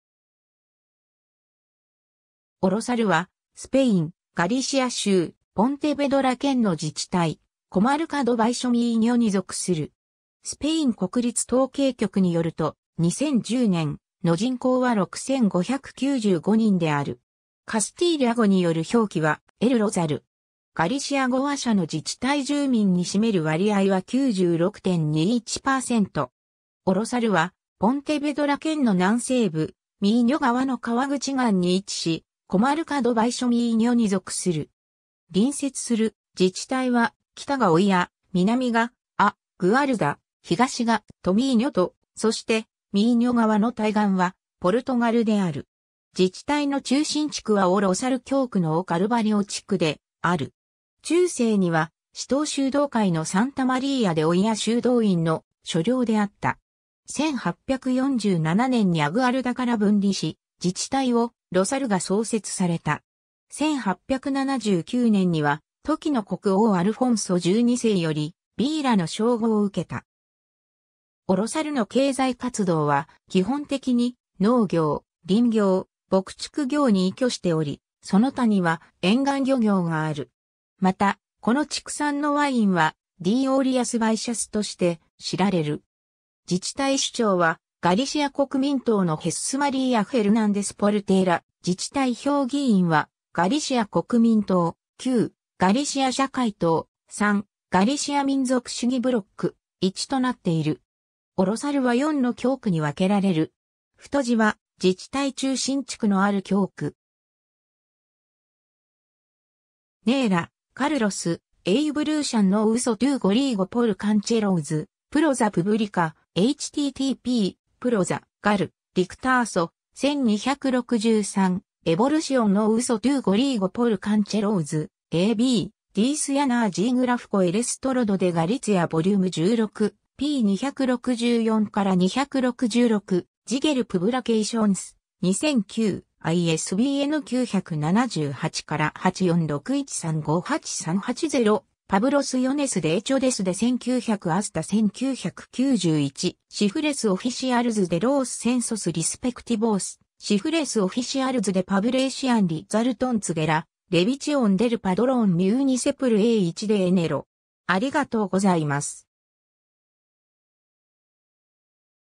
オ・ロサル
オ・ロサル（O Rosal）は、スペイン、ガリシア州、ポンテベドラ県の自治体、コマルカ・ド・バイショ・ミーニョに属する。
スペイン国立統計局によると、2010年の人口は6,595人（2009年：6,570人）である。
カスティーリャ語による表記はEl Rosal。
 オ・ロサルはポンテベドラ県の南西部、ミーニョ川の河口岸に位置し、コマルカ・ド・バイショ・ミーニョに属する。
隣接する自治体は、北がオイア、南がア・グアルダ、東がトミーニョと、そしてミーニョ川の対岸はポルトガルである。
自治体の中心地区はオ･ロサル教区のオ・カルバリオ地区である。
 中世にはシトー修道会のサンタ・マリーア・デ・オイア修道院（Mosteiro de Santa María de Oia）の所領であった。
 1847年にア・グアルダから分離し、自治体オ・ロサルが創設された。
1879年には、時の国王アルフォンソ12世より、vilaの称号を受けた。
 オ・ロサルの経済活動は基本的に、農業、林業、牧畜業に依拠しており、その他には沿岸漁業がある。
また、この地区産のワインはD.O.リアス･バイシャスとして、知られる。
 自治体首長はガリシア国民党（PPdeG）のヘスス・マリーア・フェルナンデス・ポルテーラ（Jesús María Fernández Portela）、自治体評議員は、ガリシア国民党：9、ガリシア社会党（PSdeG-PSOE）：3、ガリシア民族主義ブロック（BNG）：1となっている（2007年の自治体選挙の結果、得票順）。
 オ・ロサルは4の教区に分けられる。
^ Neira, Carlos (ガリシア語), Evolución no uso do galego por concellos, Praza Pública., ^ a b Dicionario Xeográfico Ilustrado de Galicia vol.16、p.264-266、Xeogal Publicacións、2009、ISBN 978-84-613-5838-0 ^ Poblaciones de hecho desde 1900 hasta 1991. Cifras oficiales de los Censos respectivos. ^ Cifras oficiales de población resultantes de la revisión del Padrón municipal a 1 de enero.